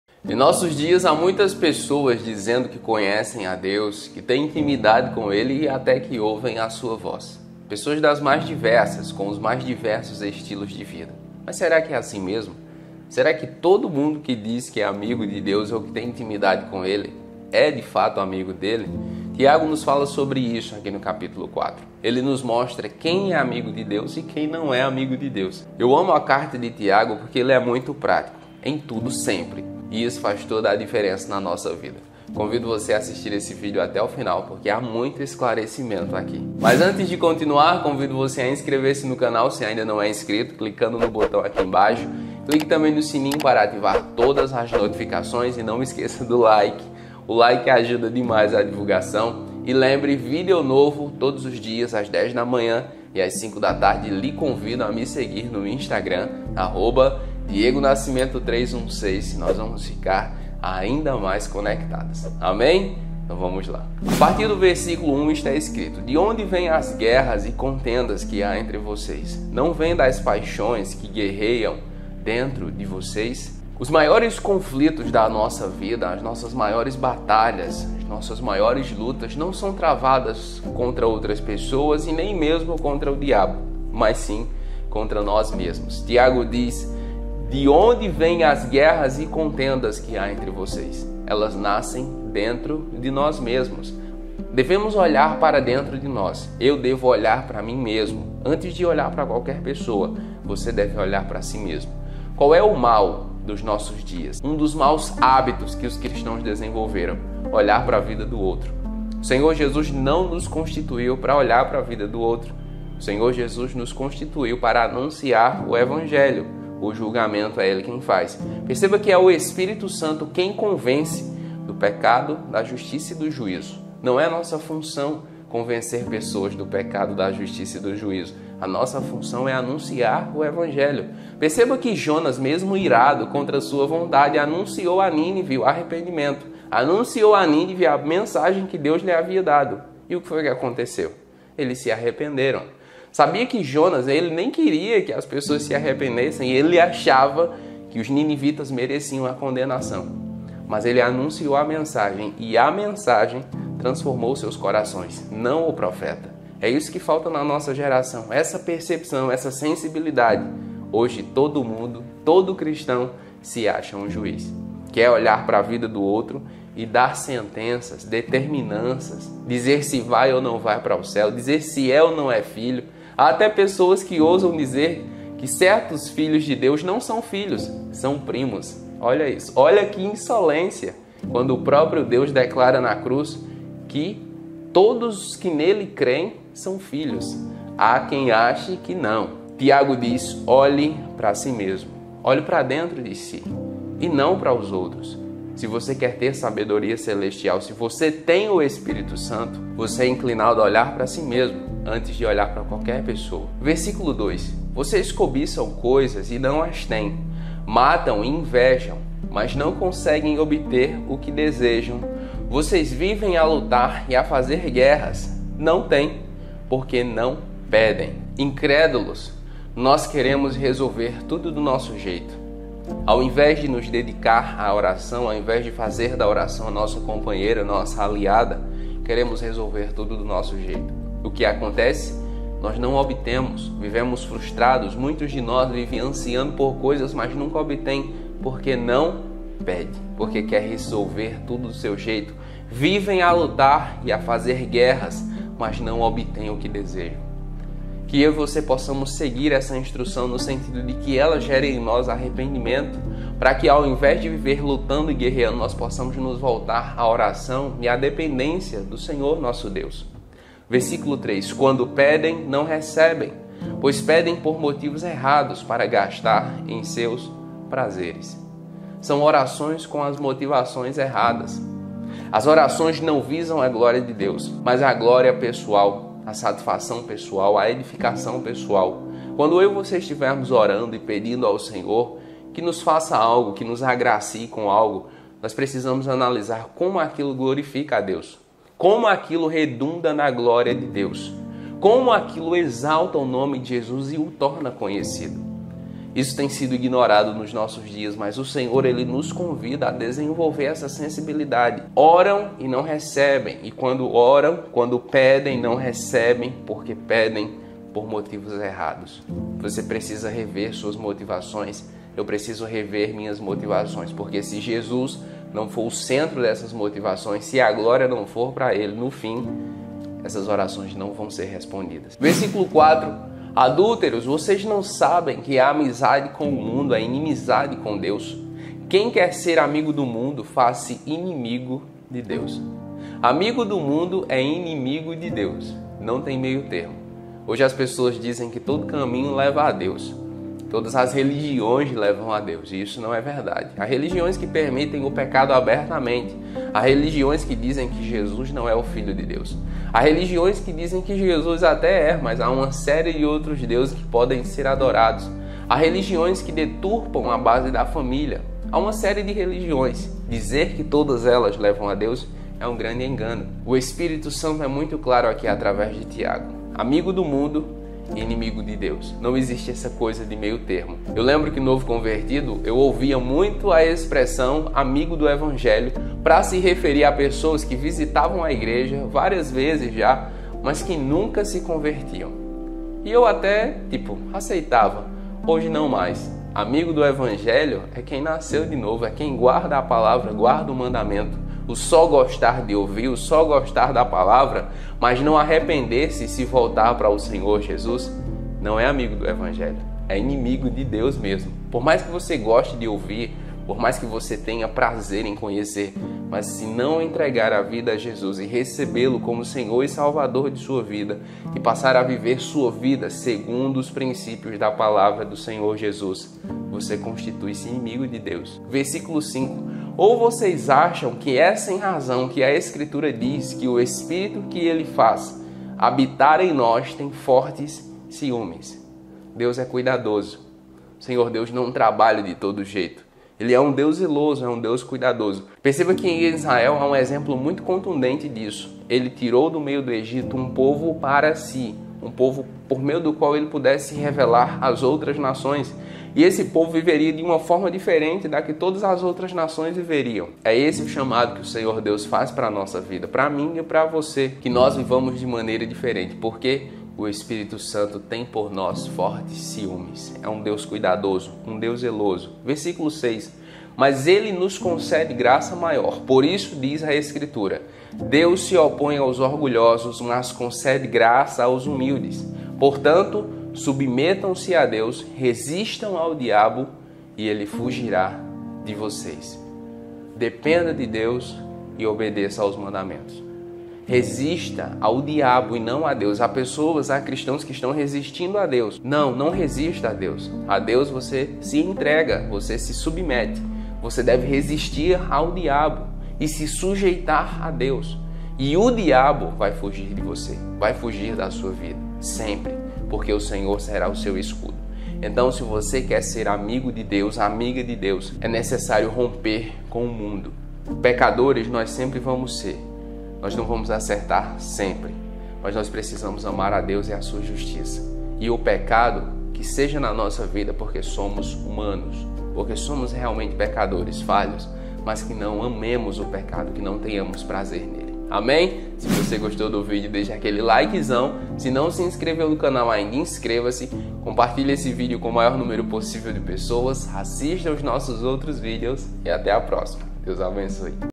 Tiago 4 Estudo: AMIGO DO MUNDO E INIMIGO DE DEUS (Bíblia Explicada)
Em Tiago 4, aprendemos qual é a raiz das guerras, discussões, desentendimentos e intrigas que há na humanidade, são os desejos malignos do coração.